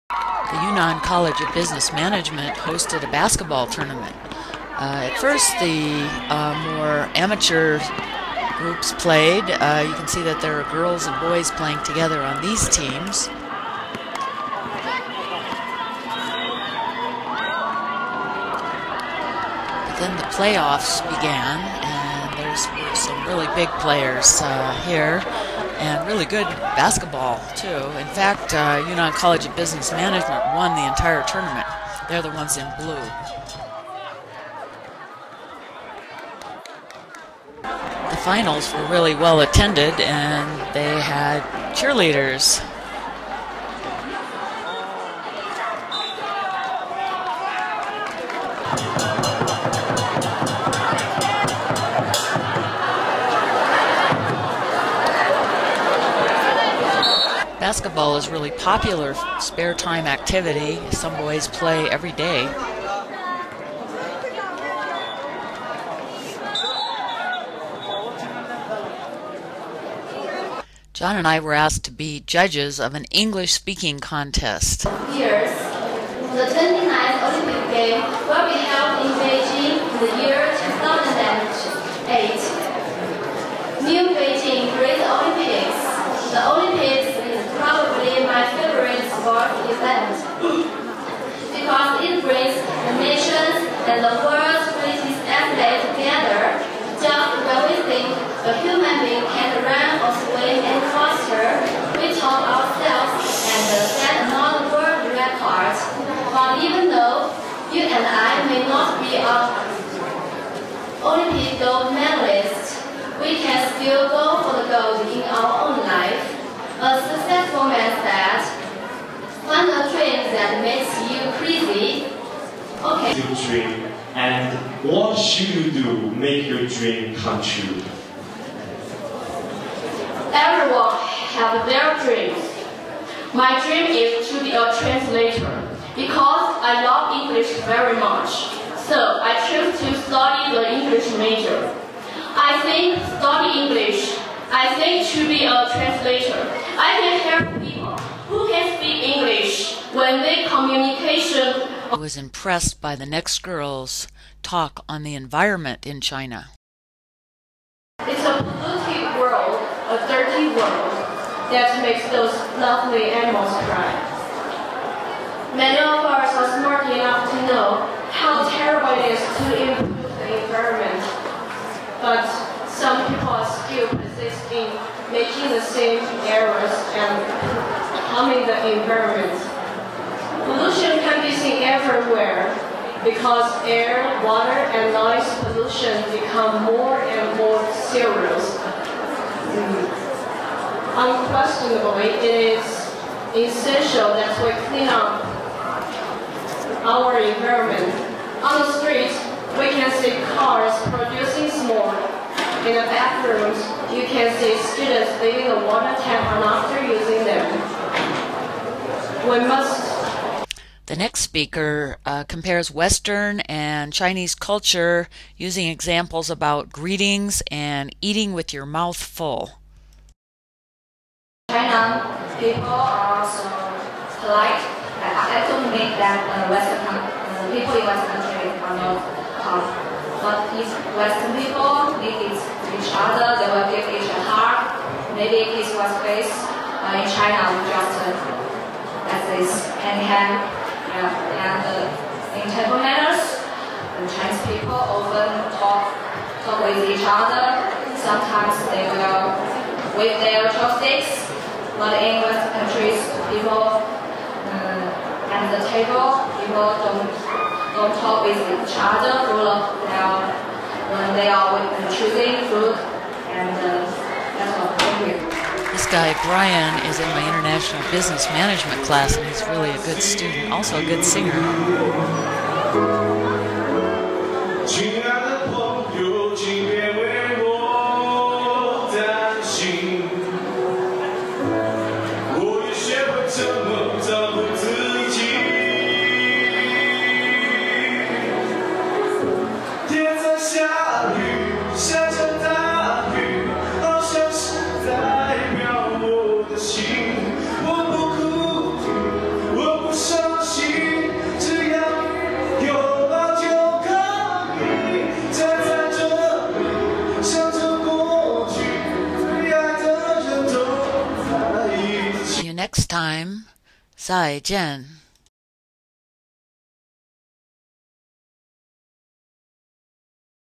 April at Yunnan College of Business Management
April means basketball tournament and English speaking contest.